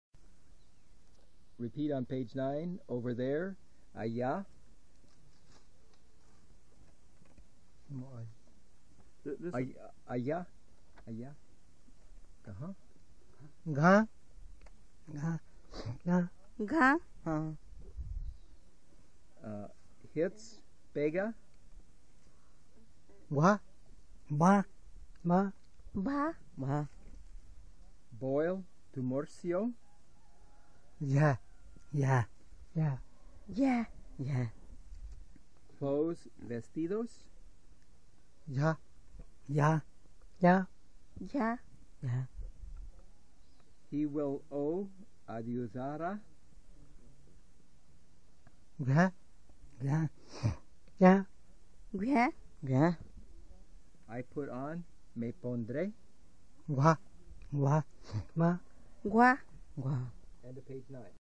Jalapa De Díaz Mazatec audio: maj_word-list_1993_40
Date: 4/5/1993

Contributor: Augustin Cruz Martinez, Bartolo Cruz (Augustin's uncle), Esteban Cruz (Augustin's father), Olga Lopez Carera, Rosalino Martinez